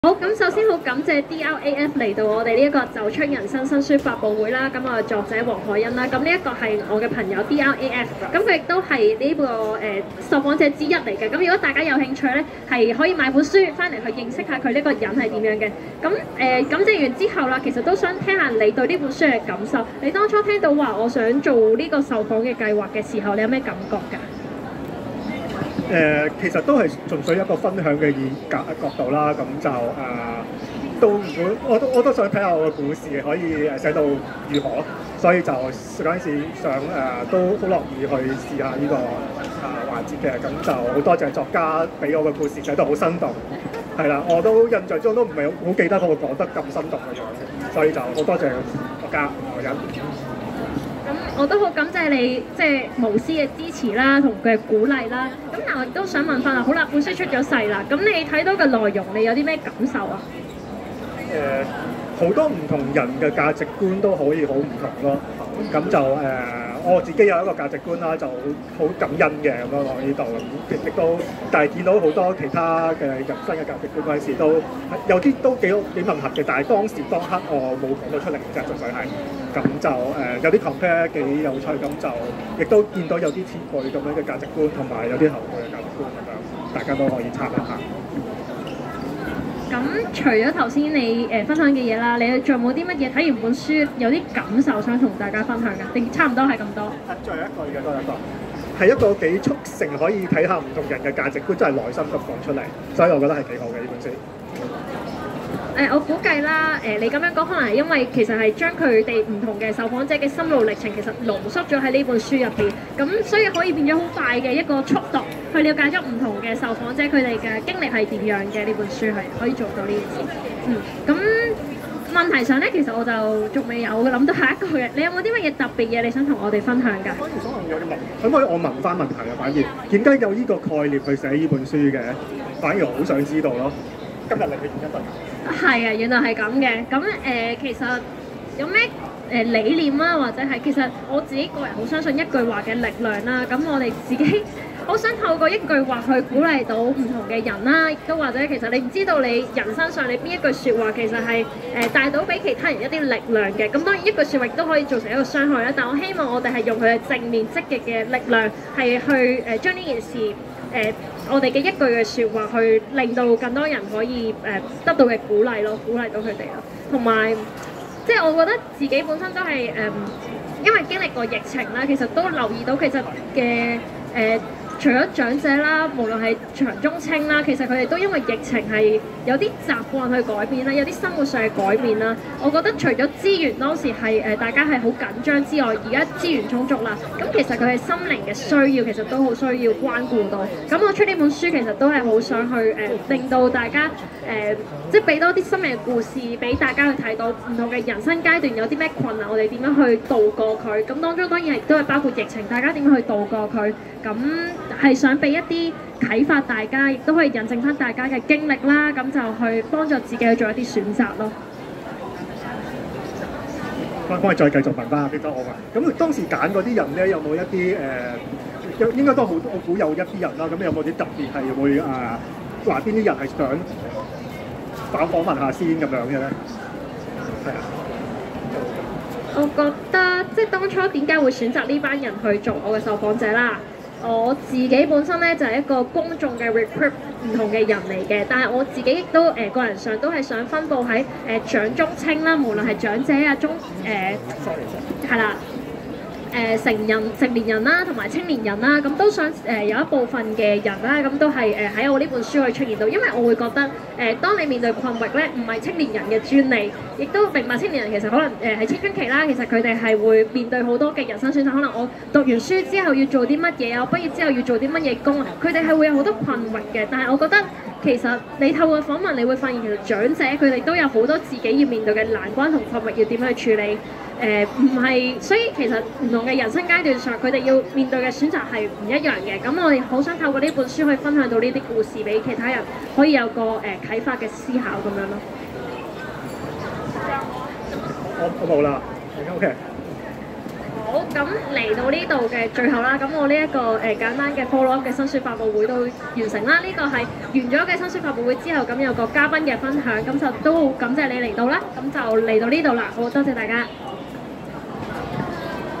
[ 2023香港書展 X 《走出人生》作家講座］黄凱昕的新書創作分享會 part 2
[ 2023香港書展 ］
作者：黄凱昕
書名：《走出人生》
地點：書展超媒體出版社攤位1B-E23
現身「超媒體岀版社」的新書發佈會
作者黄凱昕小姐暨新耆士實驗室創辦人在書中以個人自身經歷和其他30位不同受訪者的分享當中，通過一個又一個的故事，以生命影響生命，由重新認識自己出發與讀者同行踏步走出困境。